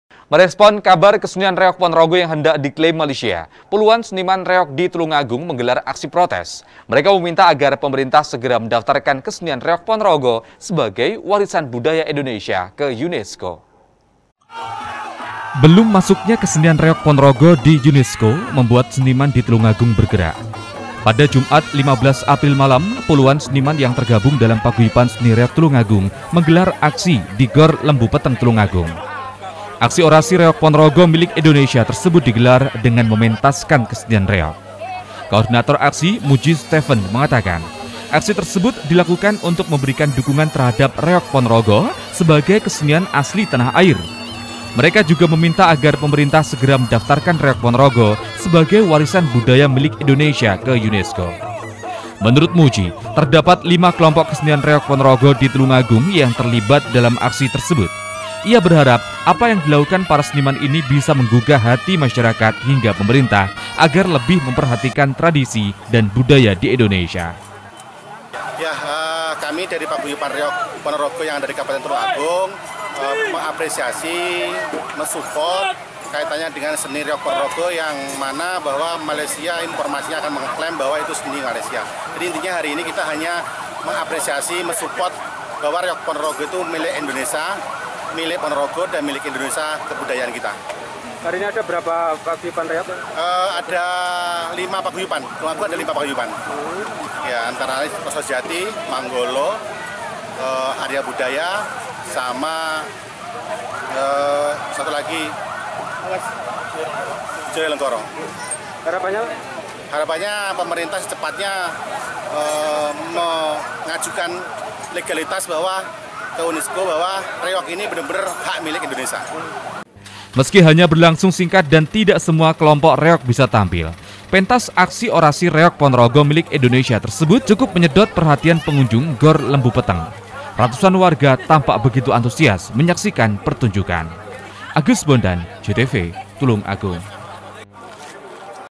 TULUNGAGUNG - Hendak Diklaim Malaysia, Seniman Gelar Aksi
Merespon kabar kesenian reog ponorogo yang hendak diklaim malaysia puluhan seniman reog di tulungagung menggelar aksi protes. Mereka meminta agar pemerintah segera mendaftarkan kesenian reog ponorogo sebagai warisan budaya indonesia ke unesco.